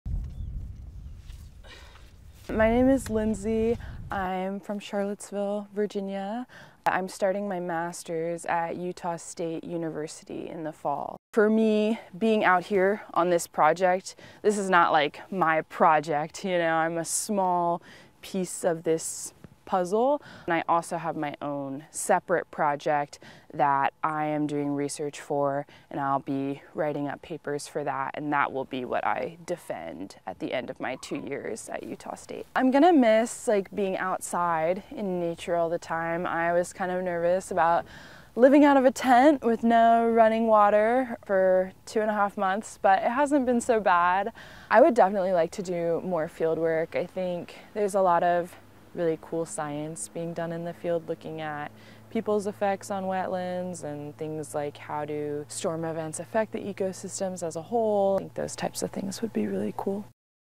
Lindsay Miller, Masters Student
Lindsay Miller, Masters Student at Utah State University, describes her new experience in the YK Delta.